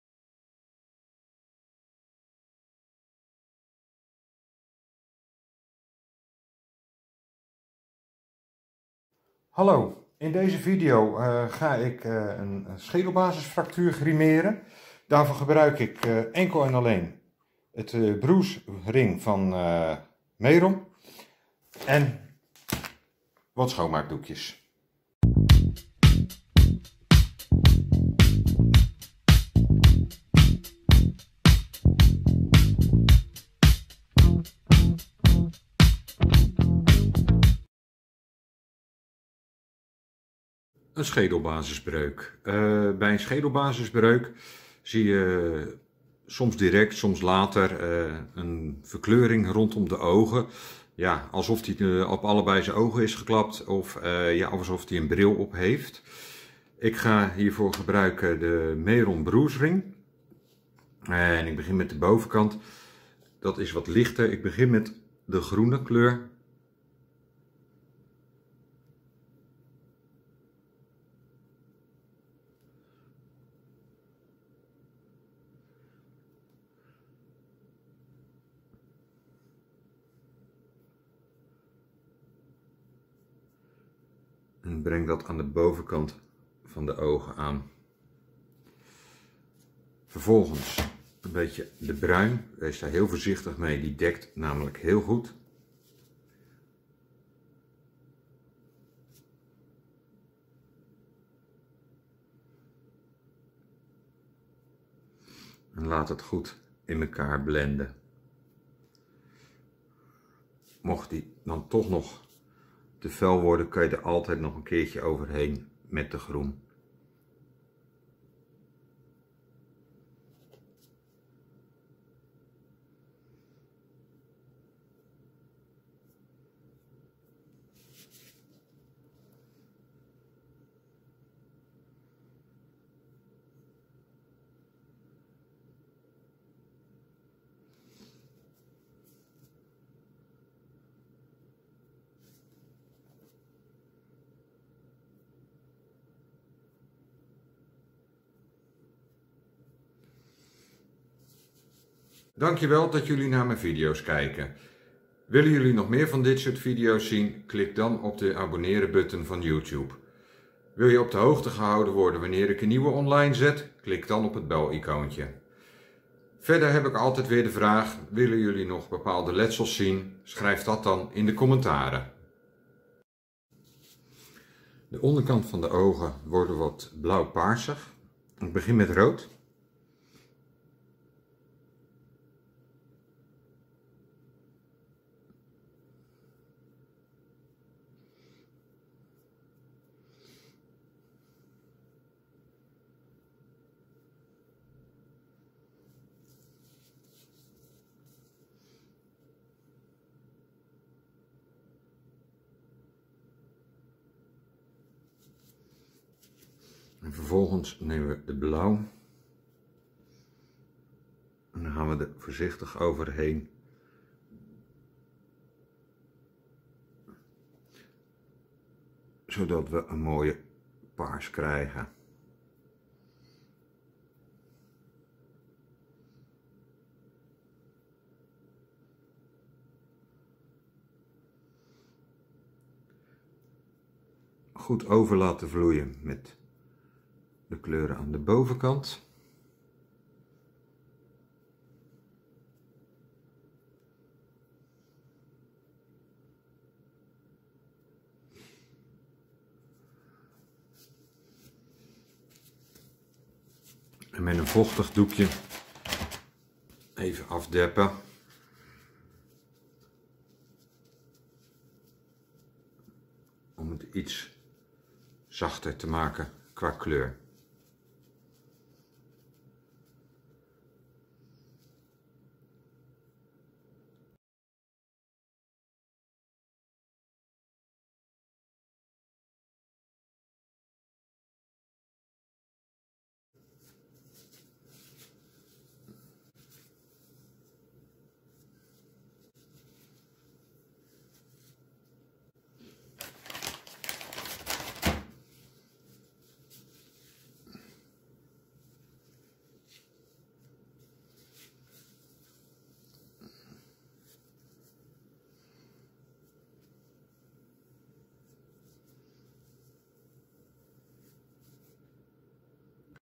schedelbasis fractuur
In deze video grimeer ik de uiterlijke verschijnselen van een schedelbasis fractuur

LOTUSslachtoffer